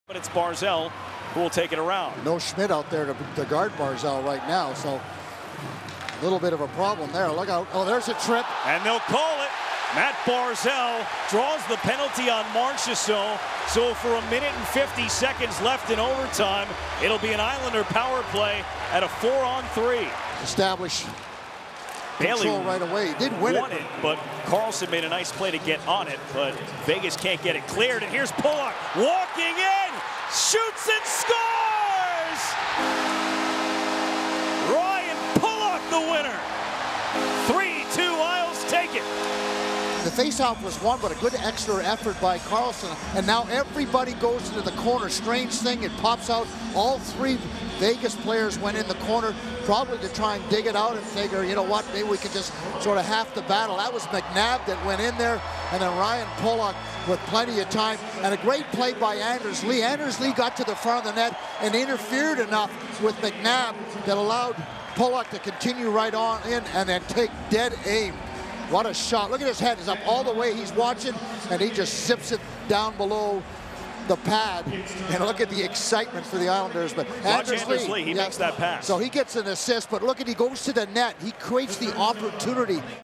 Pulock's snipe gives Isles win over Vegas in OT
Ryan Pulock skates inside the slot and snipes a wrist shot past Malcolm Subban to seal the Islanders' 3-2 win in overtime

Feeling social?